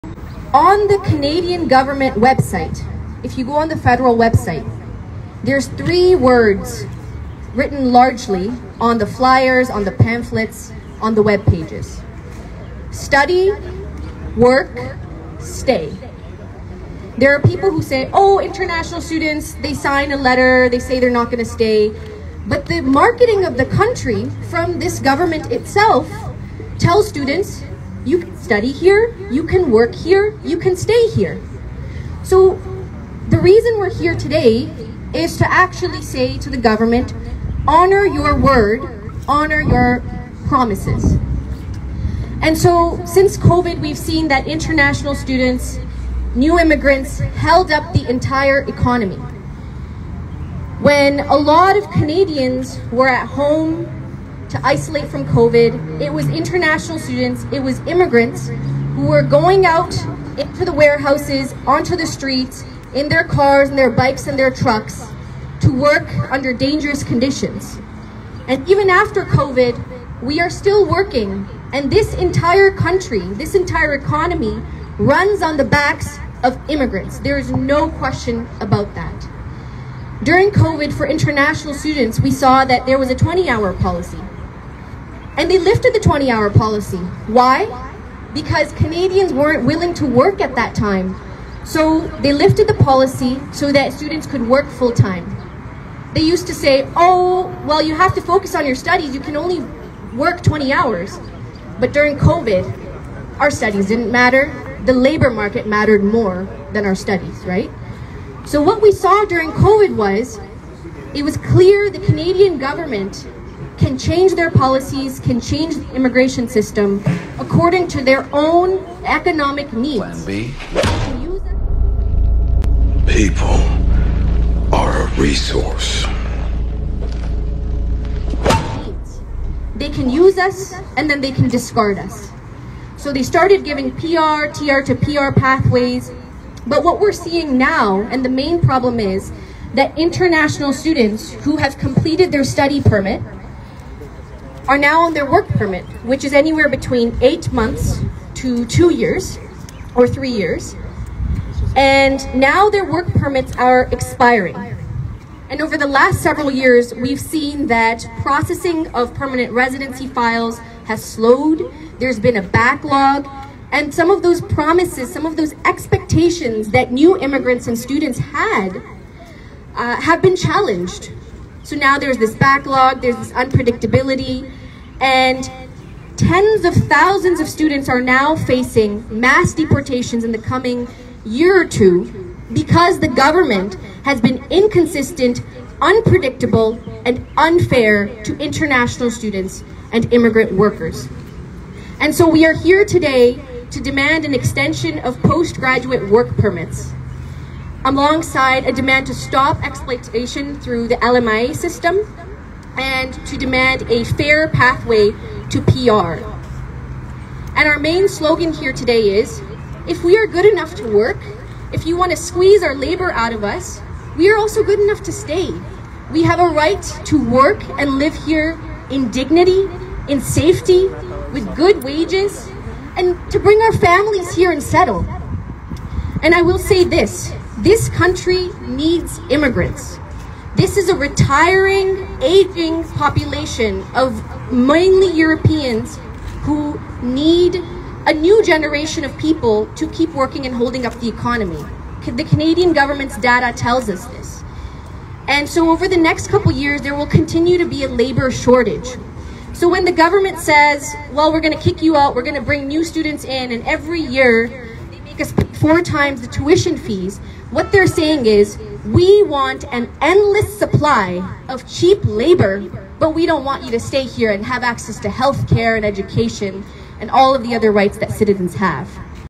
It Begins: Mass Immigration Protest in Ontario
What you're hearing in this clip is an outtake from an immigrant rally in Brampton.

The purpose of the rally is to convince the government to roll over on its own policies and the grant temporary foreign workers citizenship simply just because.

A contract is a contract.